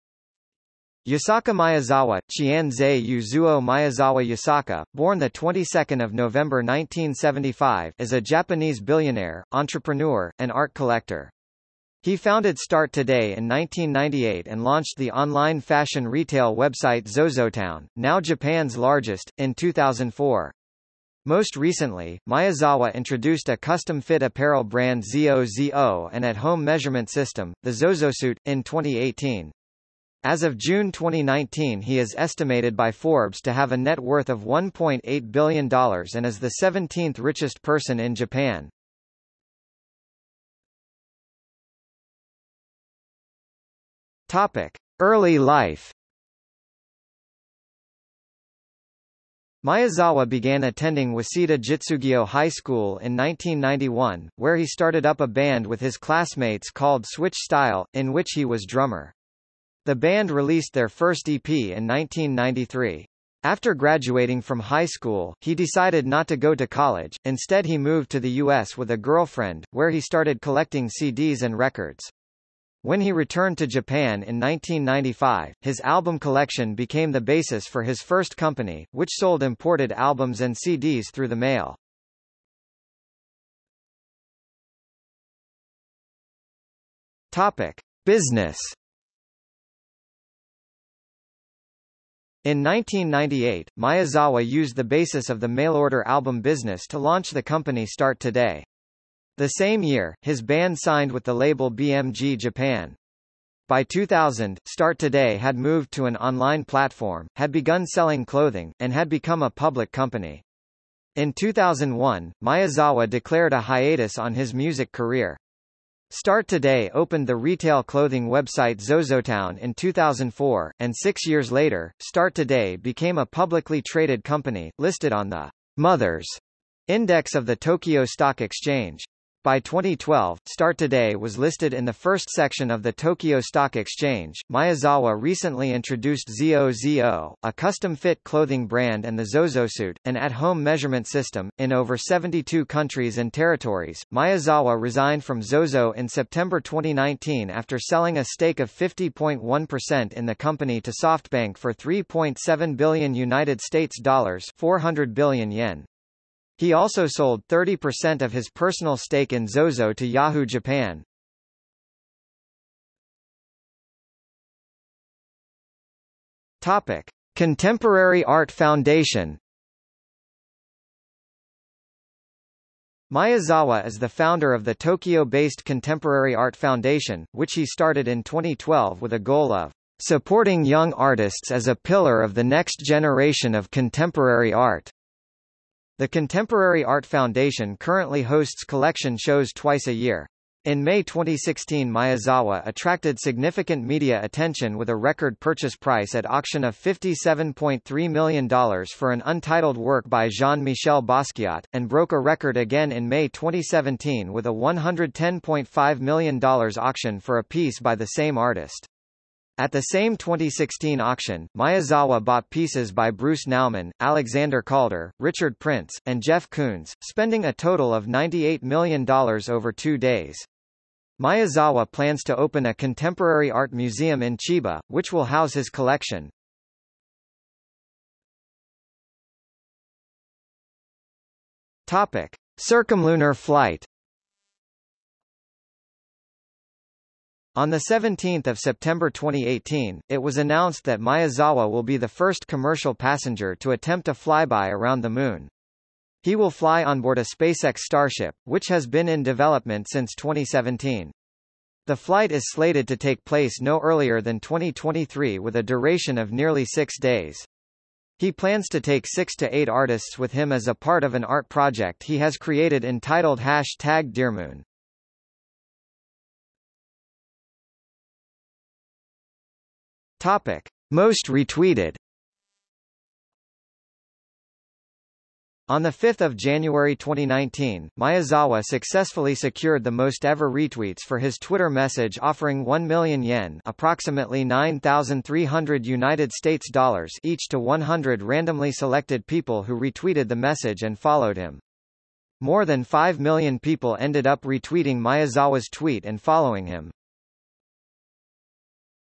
Yusaku Maezawa | Wikipedia audio article
00:00:40 1 Early life
00:01:23 2 Business
00:02:43 3 Contemporary Art Foundation
00:03:48 4 Circumlunar flight
00:04:26 5 Most retweeted

- Socrates

SUMMARY
Yusaku Maezawa (前澤 友作, Maezawa Yūsaku, born 22 November 1975) is a Japanese billionaire, entrepreneur, and art collector. He founded Start Today in 1998 and launched the online fashion retail website Zozotown, now Japan's largest, in 2004. Most recently, Maezawa introduced a custom-fit apparel brand ZOZO and at-home measurement system, the ZOZOSUIT, in 2018.  As of June 2019 he is estimated by Forbes to have a net worth of $1.8 billion and is the 17th richest person in Japan.